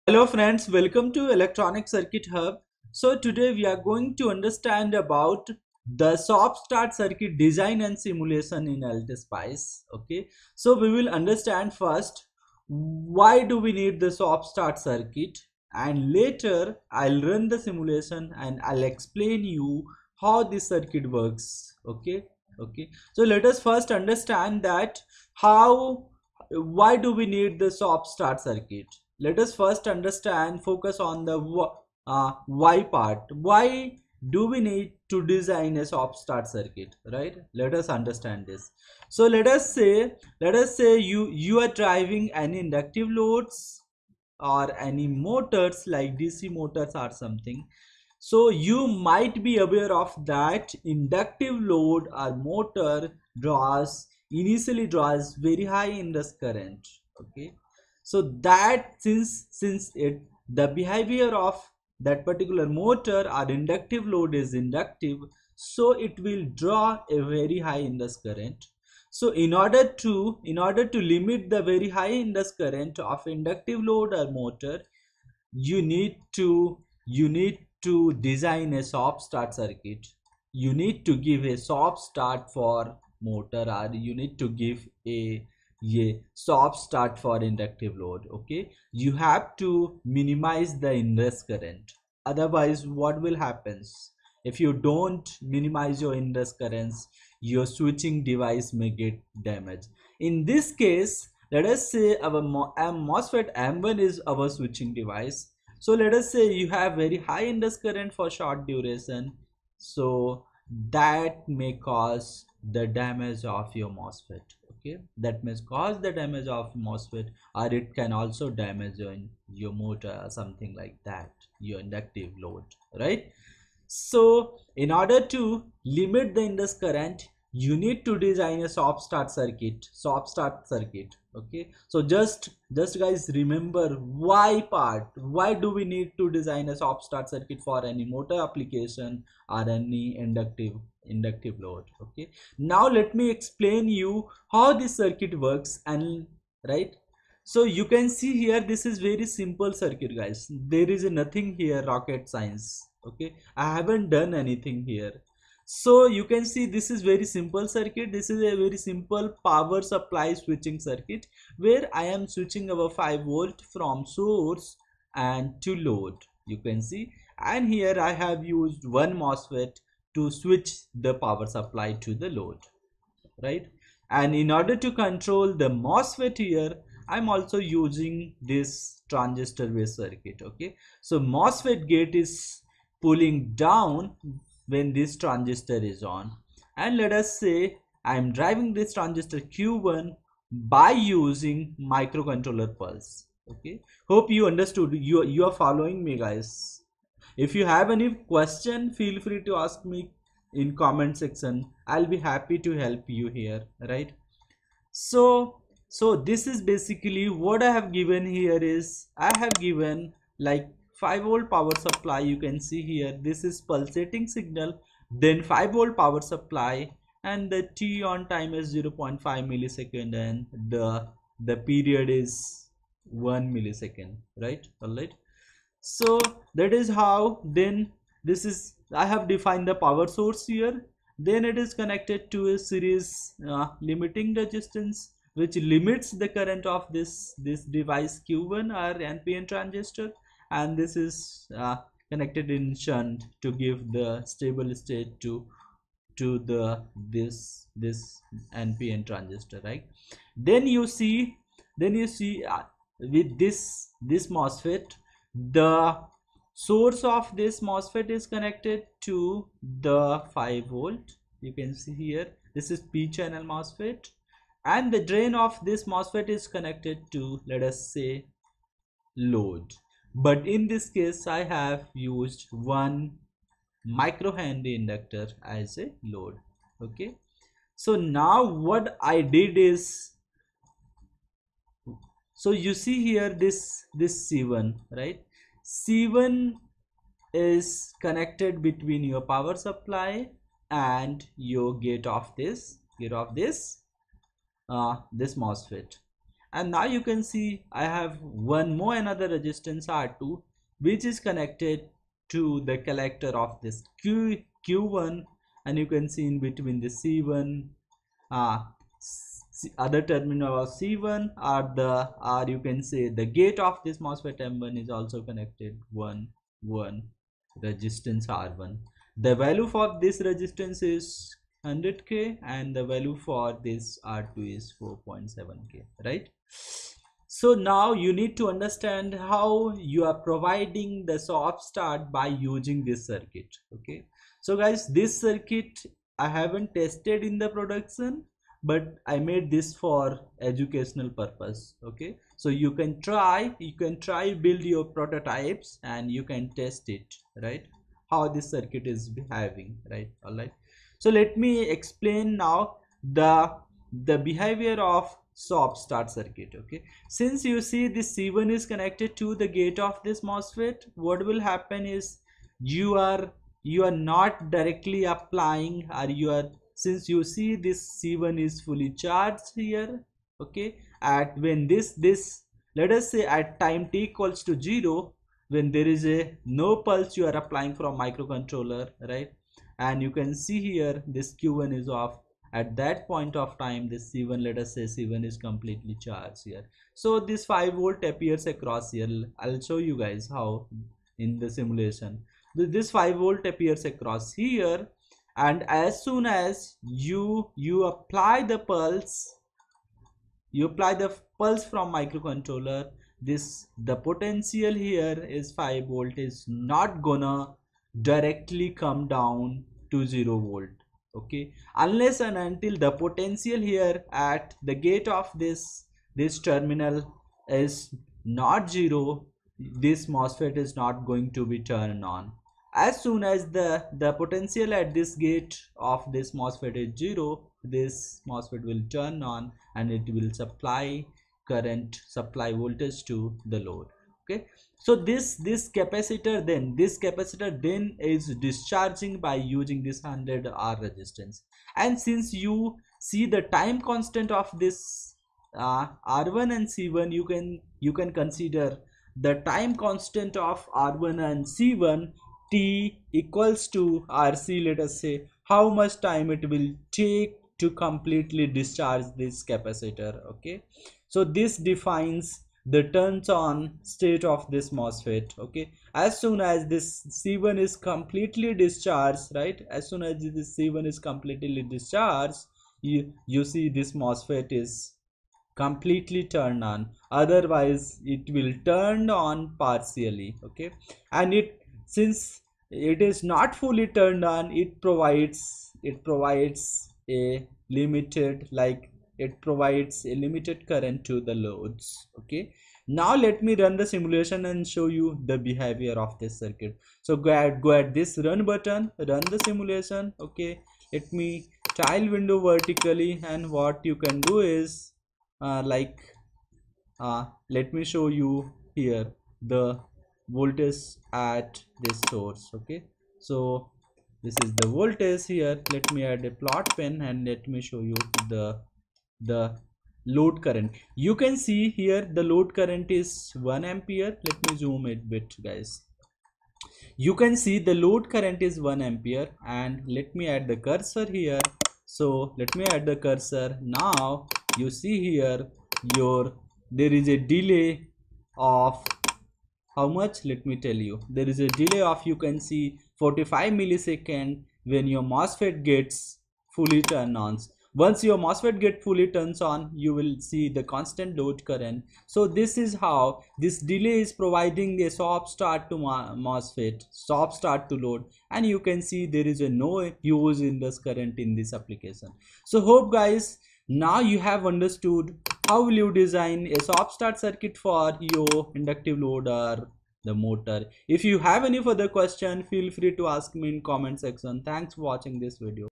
How to Simulate a Soft Start Circuit in LTSpice | Full Tutorial
This video explains about the soft start circuit design using ltspice.
Application of this circuit is to drive motor or inductive loads.